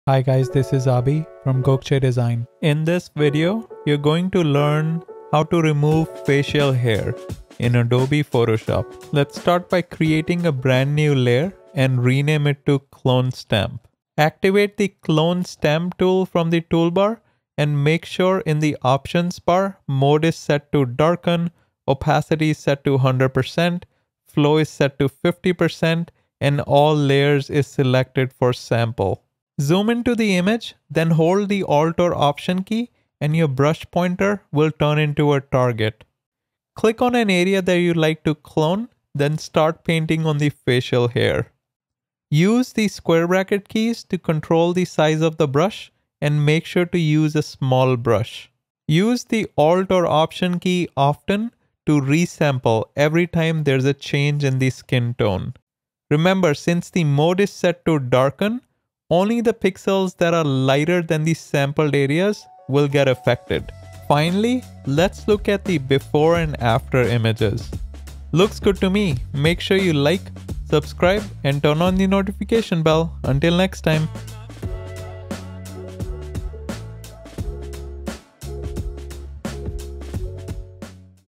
How To Remove Facial Hair In Photoshop (1 Min) | Remove Beard, Stubble
In this tutorial, you'll learn how to remove facial hair in Photoshop using the Clone Stamp Tool & Darken Blend Mode.